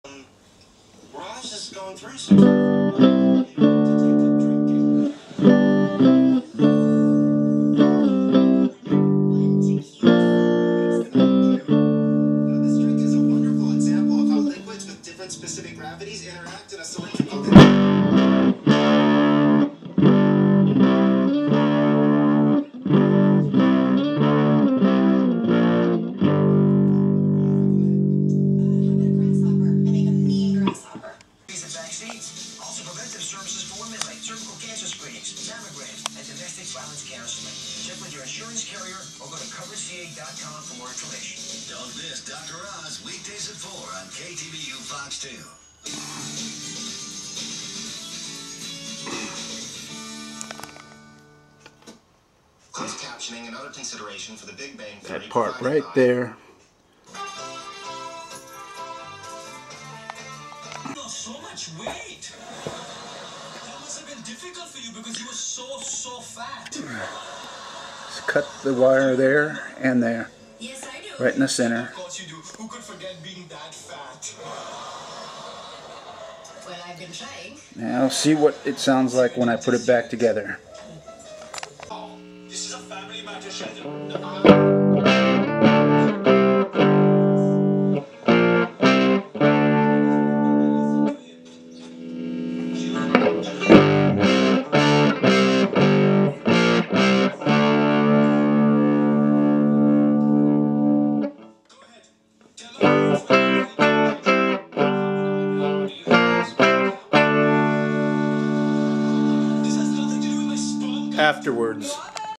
Epiphone Valve Junior The JP1 Mod
The sound before the one cut modification
I saw this mod on youtube from SoundGarageTales.  So decided to try it
The sound after
Yes it is brighter but also noisier, fender stratocaster  single coil neck pickup the amp also  humms now